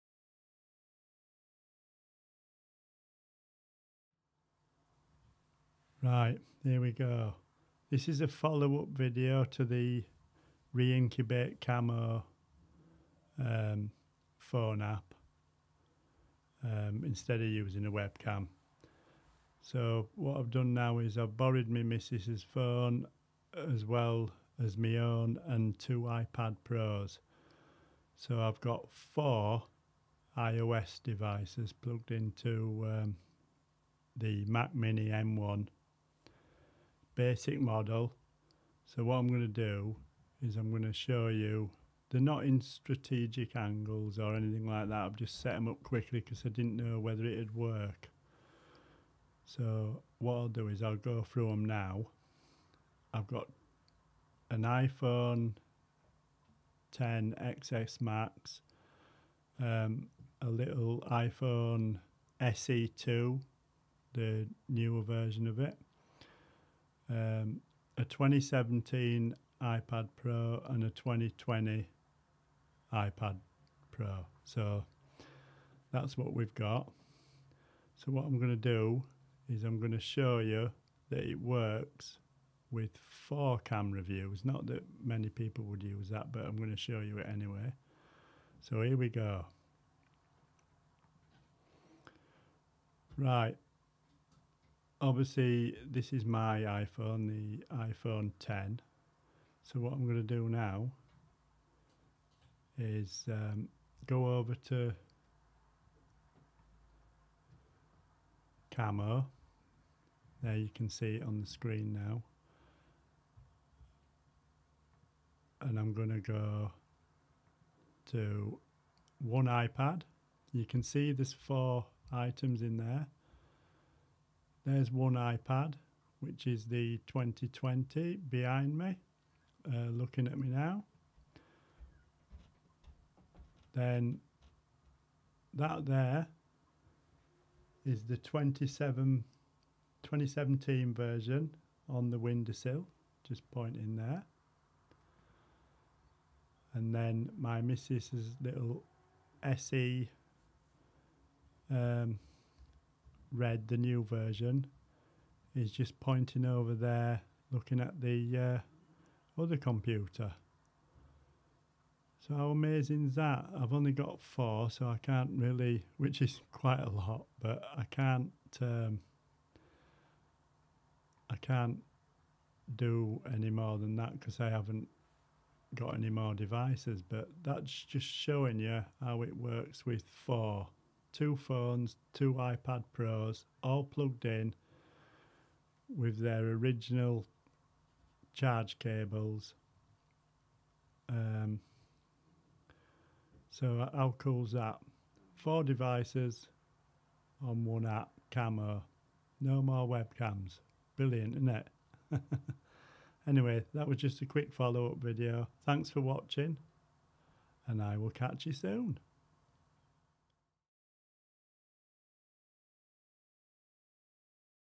reincubate camo app running 4 cameras,2 iPhones+2 iPads (awesome)
this was a follow up video for running 4 iOS devices ie 4 cameras
in one app works floorless,once lockdown is over I'm going to try 5 once
friends are aloud over